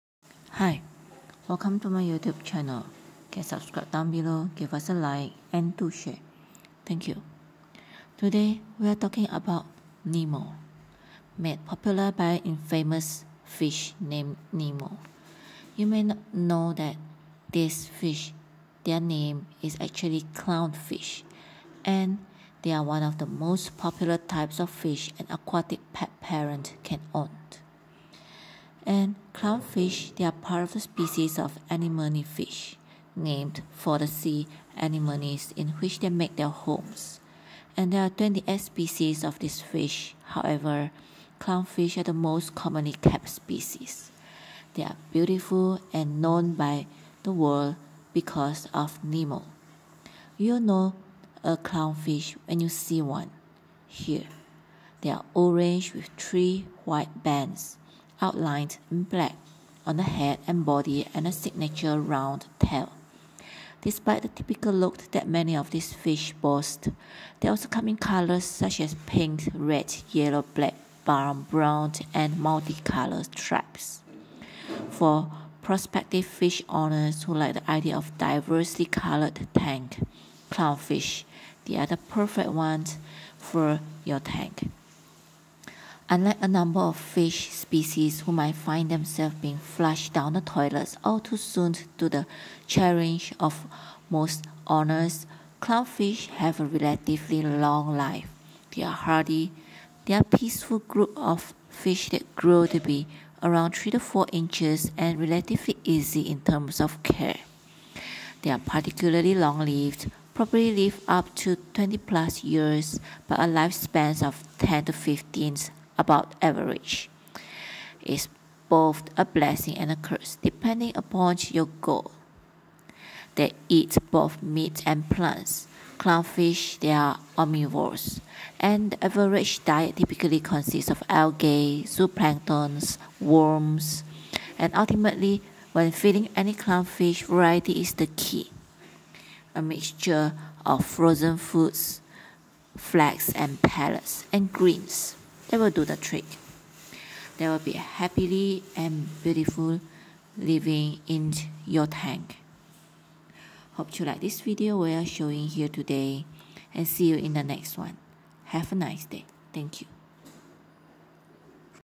My Favourite NEMO! Do You Keep Clownfish in your Aquarium? They are the BEST! | Cute Pets Bonding
NEMO is finally here! Do you know it's actual name is clownfish? Nemo is just fascinating to watch, so beautiful and interesting! They are easy to care, hardy, and long lifespan, Sharing is Caring